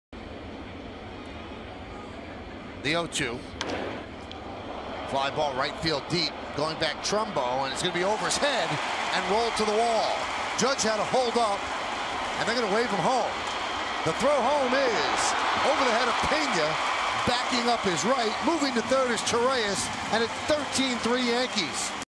BAL@NYY: Torreyes doubles home Judge in the 7th
Ronald Torreyes plates Aaron Judge with an RBI double, extending the Yankees' lead and advancing to third on Jonathan Schoop's error